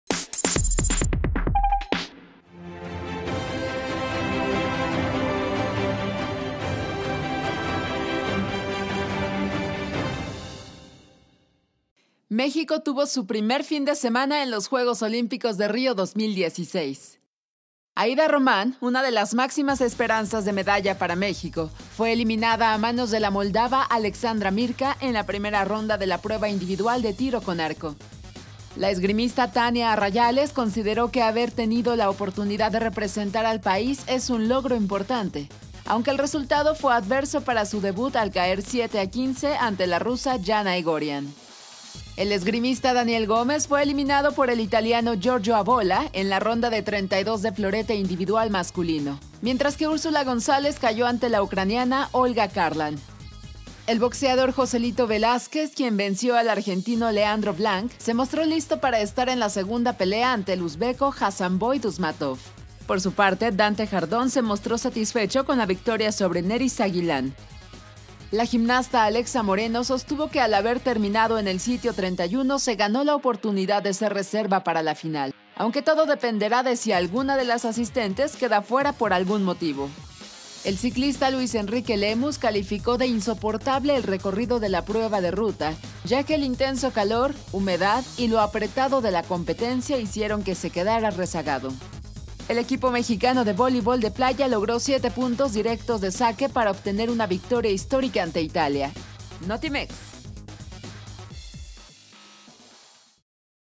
Primer fin de semana para México en Río 2016
México tuvo su primer fin de semana en los Juegos Olímpicos de Río de Janeiro